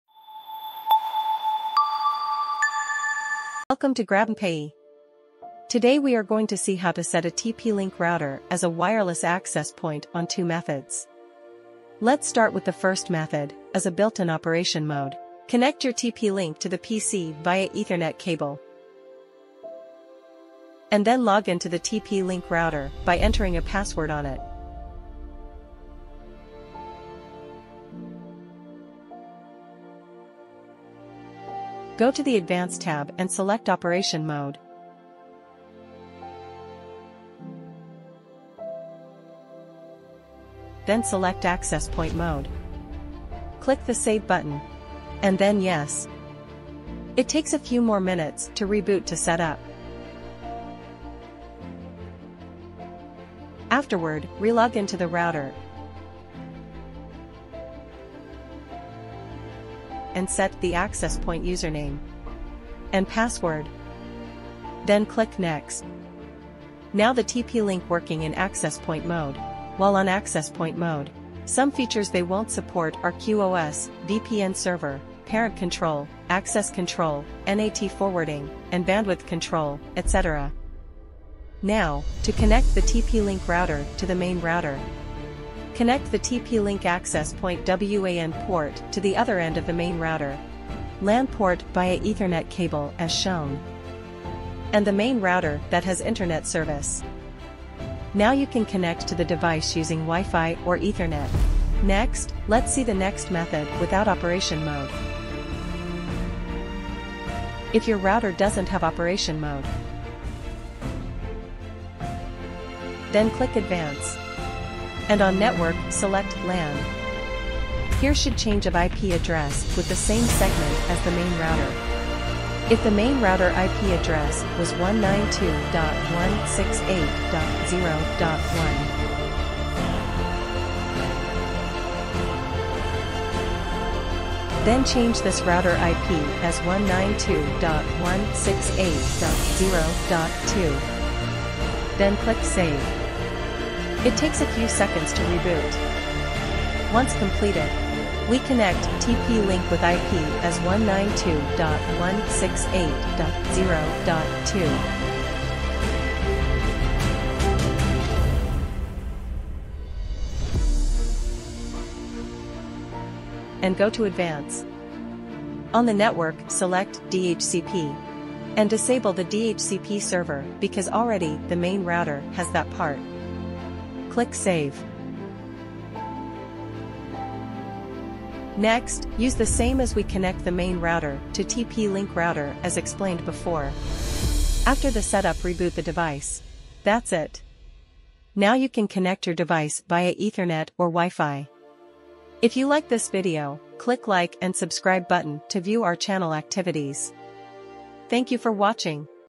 How to Configure Access Point Mode on TP-Link Archer Wi-Fi Router?
In this video, learn step-by-step how to set up Access Point Mode on your TP-Link Archer Wi-Fi Router using two simple methods, ensuring improved network coverage and connectivity.

Timestamps:-
00:05 - Introduction
00:20 - Built In Operation Mode (Method 1)
01:21 - Connectivity Diagram
01:40 - Without Operation Mode ( Method 2)

to learn more about a product or make a purchase